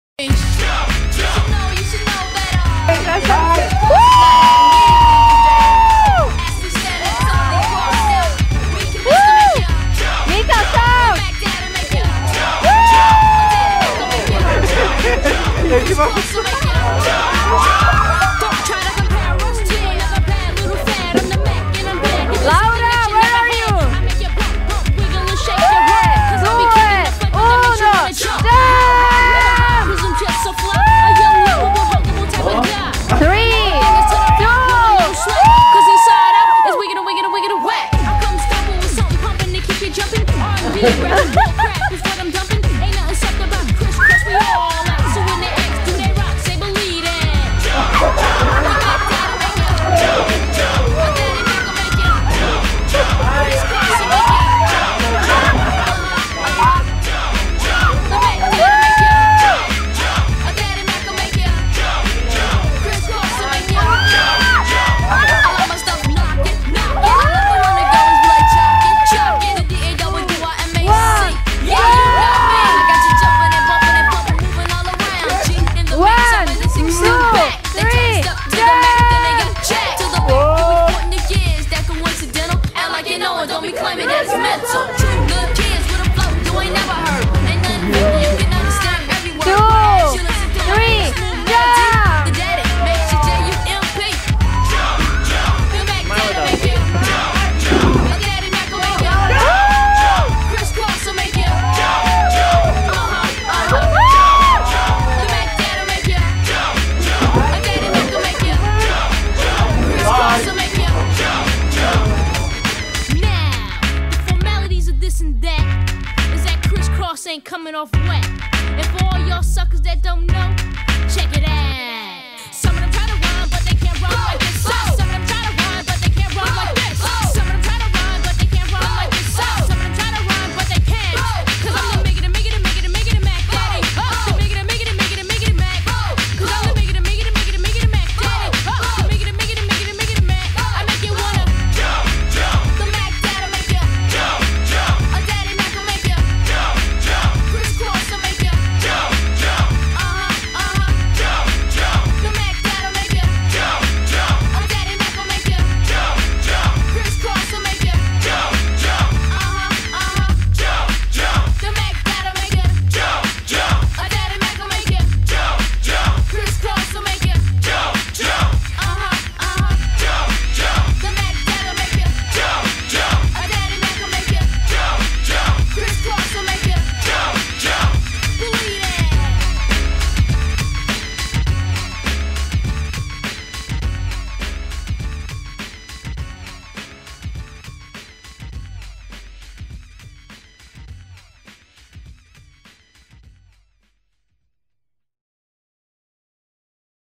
Bratislava Night Trip with Bungee jumping
7.3.2014, Night trip to Bratislava, City tour, Bungee jumping and Party